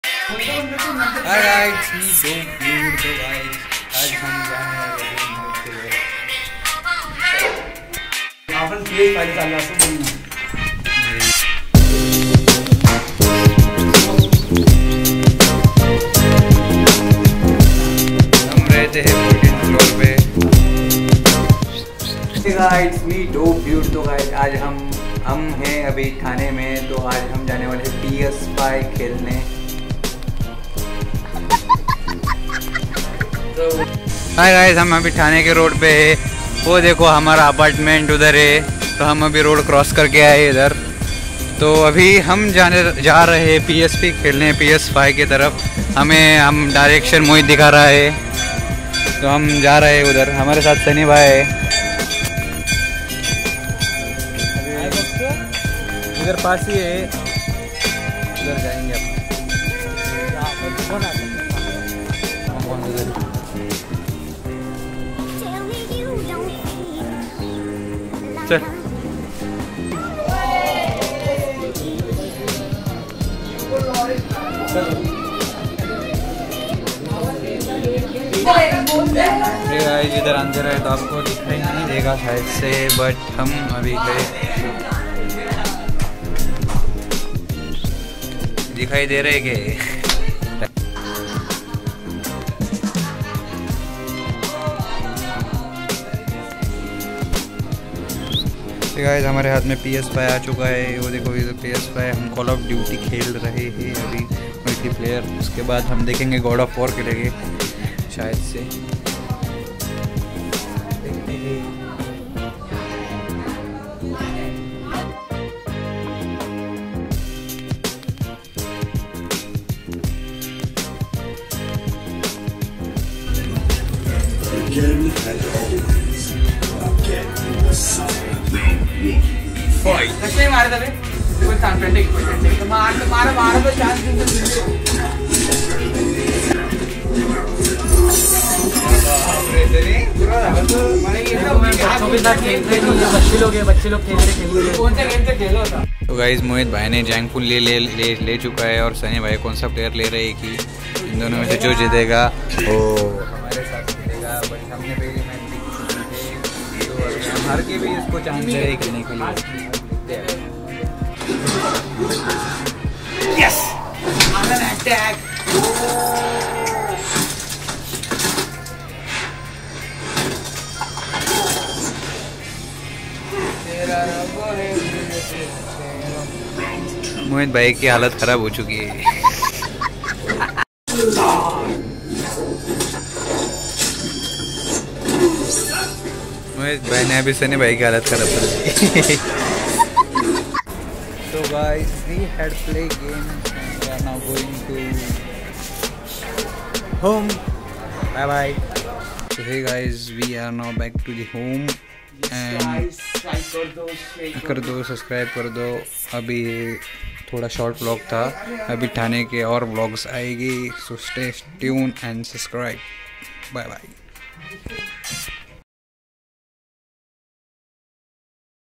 PS5 - THANE | VLOG 20 | DOPEDUDE !
Heyy guys so today we are going to play ps5 in thane. We enjoyed a lot .
DOPEDUDE !